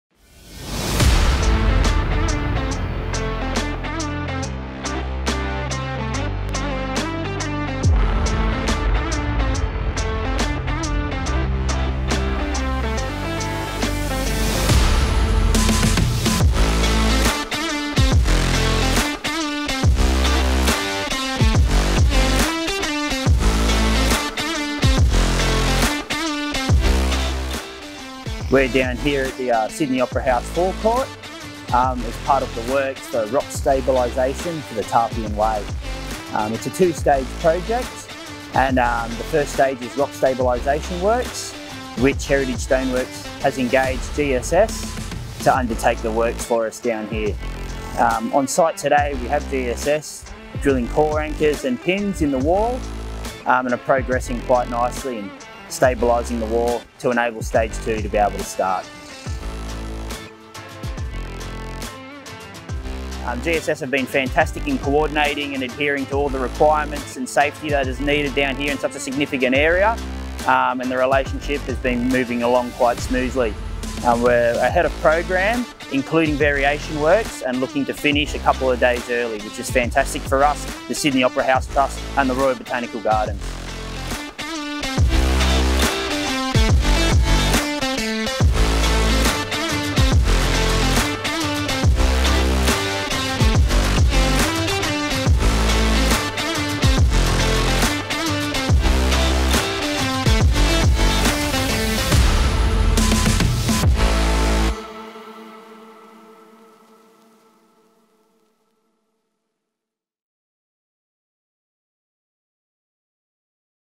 GSS - Opera House Courtyard Works
Film and photography of the Ground Stabilisation Services crew during a day at the Opera House Courtyard.

Film produced by goodmash video productions:

Film/Creative Director: Marco dos Santos @marcomavx